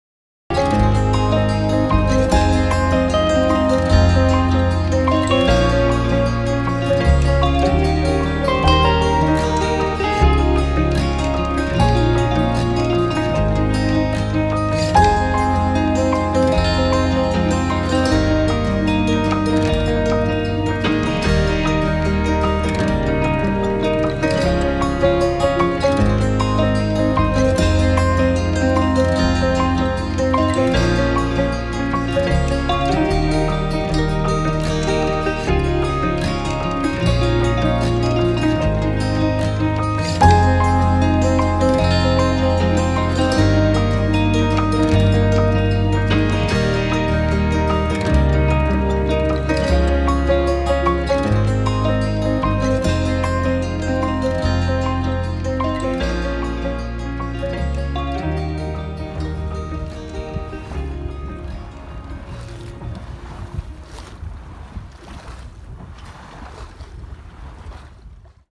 Relaxing mountain sailing (short clip)
Some nice relaxing shots on a great day of mountain sailing. Boat is a 2010 Precision 18 sailboat named "Mandatory Fun." Location is Eleven Mile State Park, Colorado.